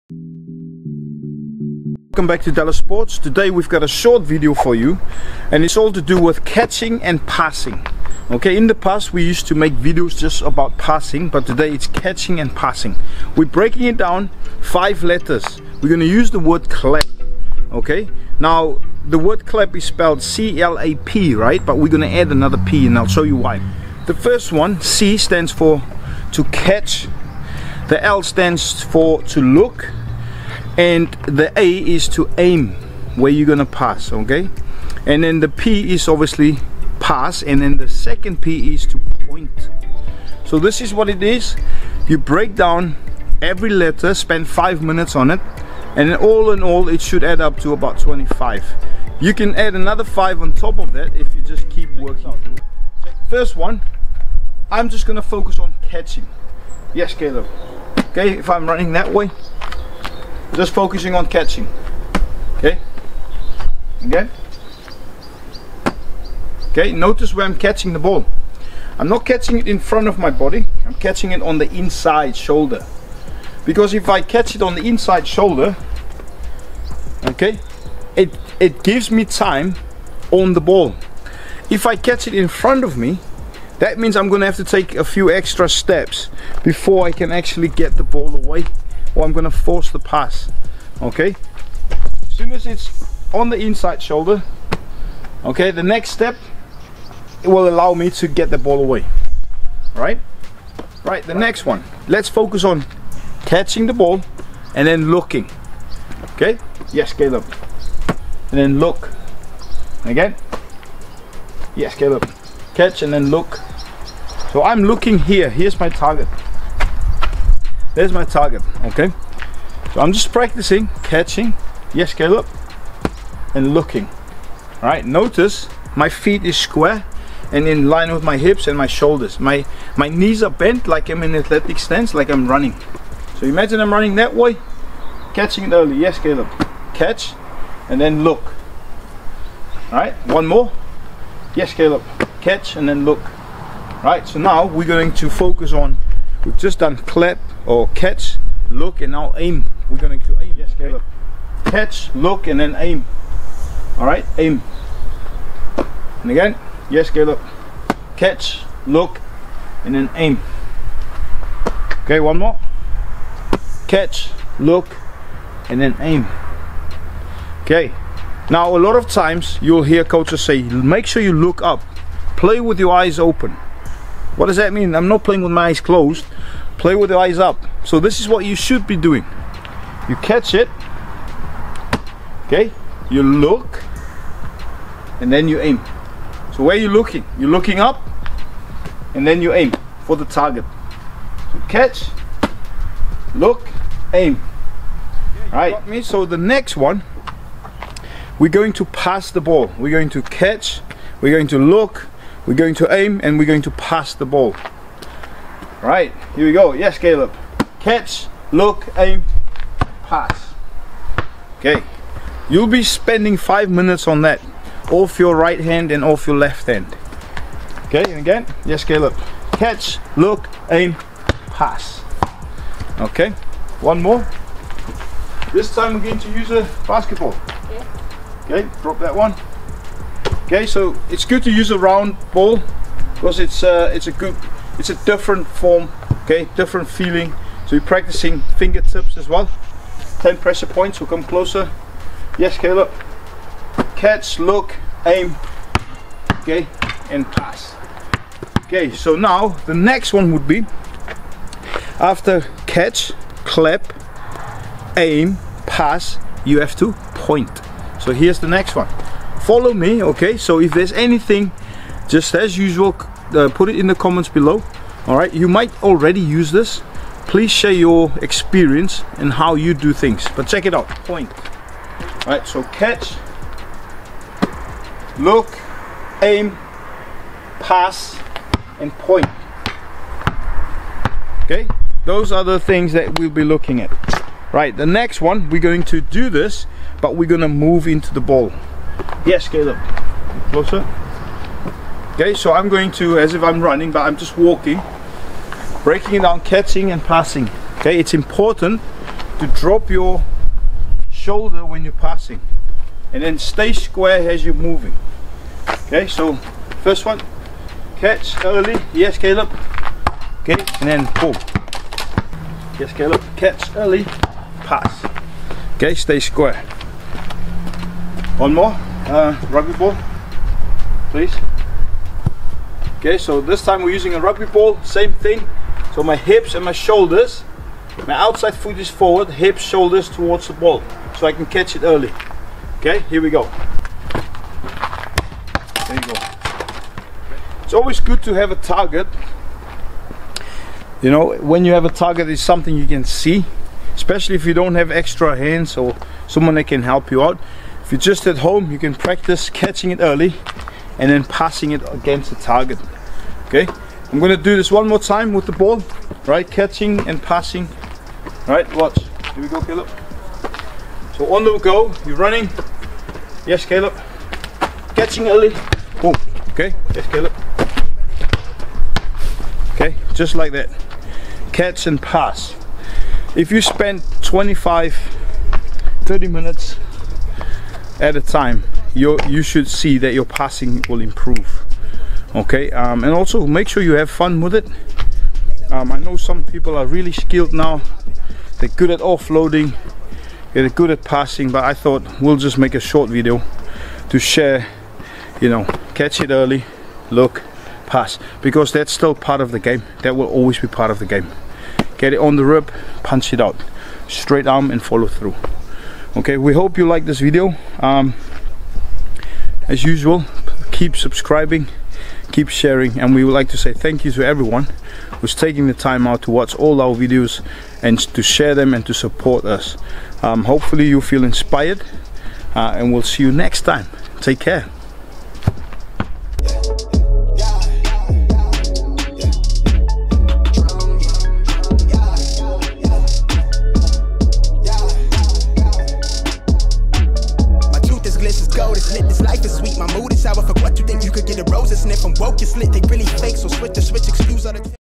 HOW TO IMPROVE CATCHING AND PASSING IN RUGBY THE CLAPP METHOD
In this video ex international scrum half and level 3 rugby coach demonstrates The CLAPP method which stands for -
Catch
Look
Aim
Point
Pass
He will be showing and breaking down how to improve your catching and passing skills.

MAKE SURE THE BELL IS ON FOR WHEN WE UPLOAD NEW VIDEOS!

As always, consult a Doctor before taking up any exercise!